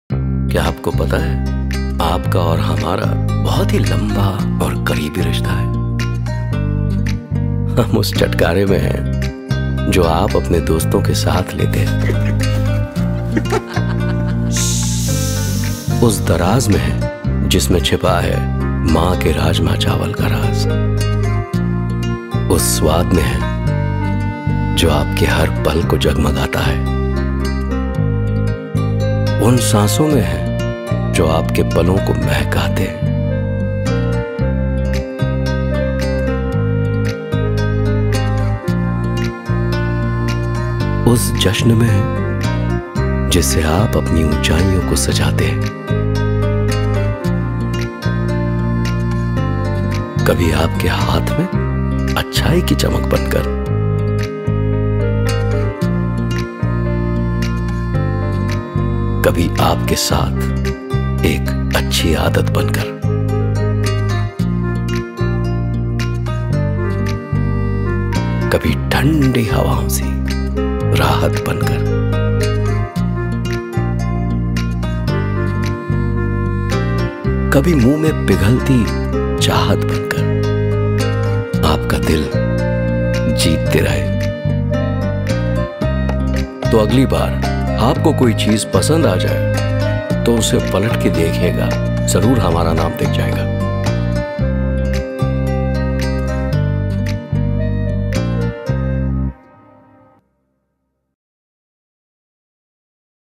DS Group Corporate Film 2.0 | Hindi I Featuring Rahul Khanna I Short Edit
Watch our latest video which captures how DS Group is touching millions of lives daily with its diverse product range as it celebrates 95 years of building iconic brands and enriching consumer experiences! The film features renowned Bollywood actor ‘Rahul Khanna’.

The film opens with him walking along a long corridor, speaking directly to the camera, "Aapka aur hamara bahut hee lamba aur kareebi Rishta hai" (Our relationship has been long and intimate). This sets the tone for a series of heartwarming moments that showcase the deep connection DS Group's products have fostered with consumers over the decades.

In a series of beautifully crafted vignettes, the film presents various scenarios where DS Group's products bring joy to people’s lives. For instance, a group of youngsters in a library, where a boy’s hidden Pulse candy sparks a playful commotion. A young man is seen cooking with Catch Garam Masala, sharing a tender moment with his mother via video call. Celebratory moments include a classy wedding scene where a man enjoys Tulsi Royal Khajoor, and a playful serenade where a boy refreshes with Pass Pass after being teased by a girl. Other scenes feature a lady in a limousine enjoying Silver Pearls before helping a stranger, a young man savoring Ksheer ghee on a parantha after a gym session, and a woman waking up to a surprise gift of LuvIt Chocolates with a birthday message.

Rahul Khanna’s voice-over enriches each scene, highlighting the joy brought by the individual brands. The film concludes with Khanna reminding viewers, “Agli baar jab aap ko koi cheez pasand aajaye, toh aap use palat ke dekhiyega…Zaroor hamara naam dikh jaayega.” (Next time when you enjoy something, make sure you take a look at the back of the pack. You will find our name there).

Social Channels: Linkedin: / dsl